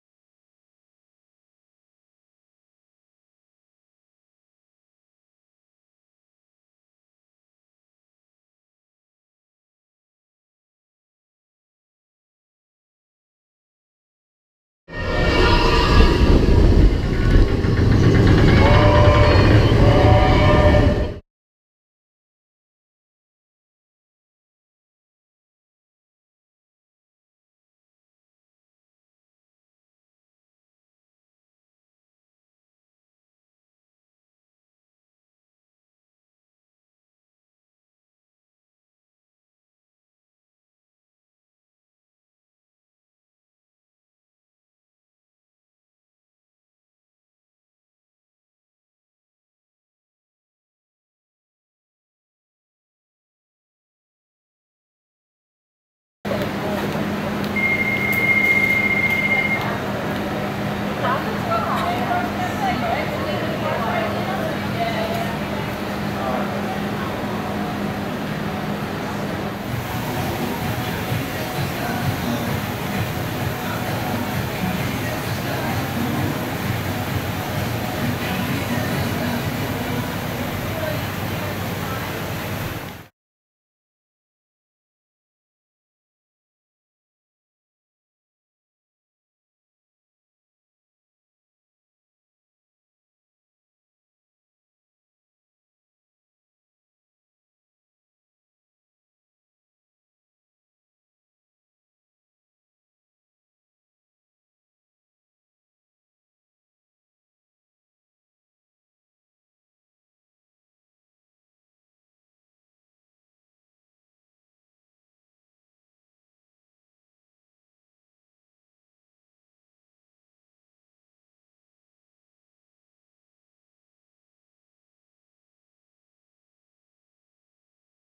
NEW Avatar Land Construction Update at Disney's Animal Kingdom Park, Walt Disney World, Pandora
An Avatar land is being built at Disney's Animal Kingdom Park, Walt Disney World. Here is a look at the construction from different angles, including from Expedition Everest. This video includes photos and video from August 4th, 2015, as well as concept art released by the Walt Disney Company.